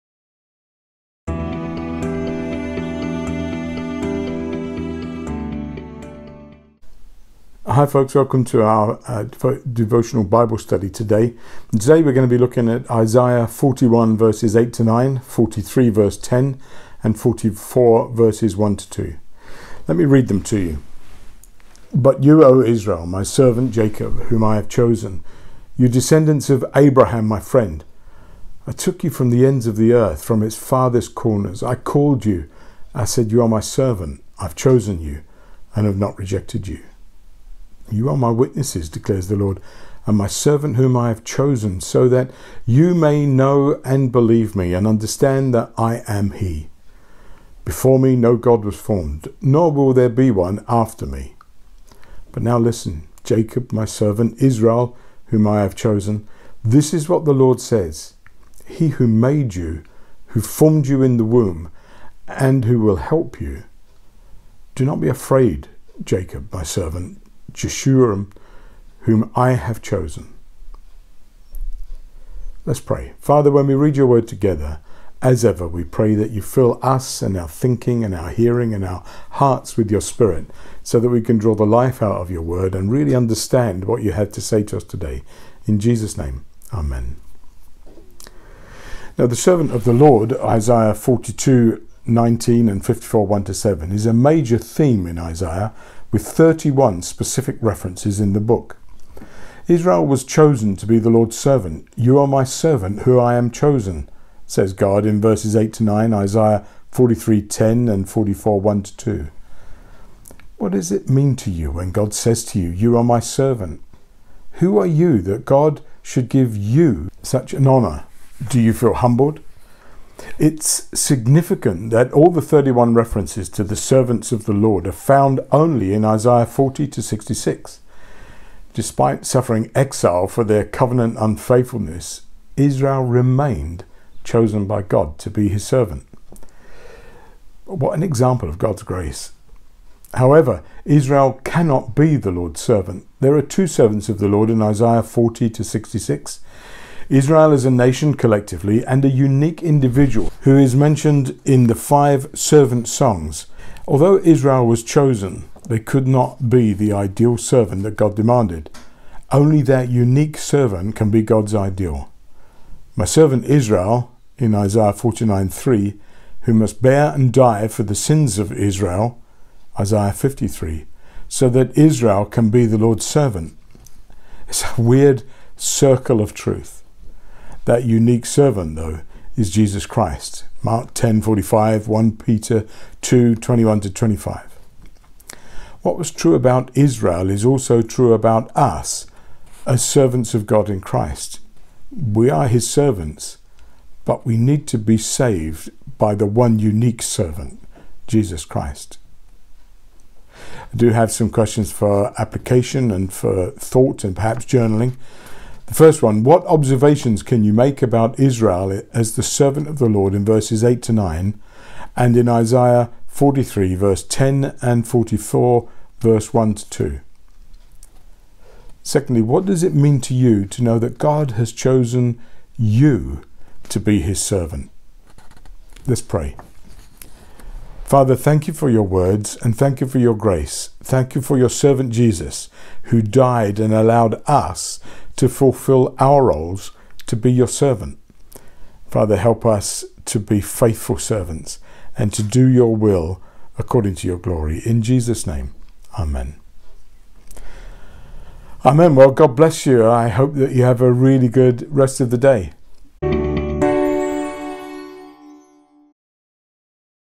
Bible Study Monday 8th March 2021 HD 720p
Dave shares a short devotional bible study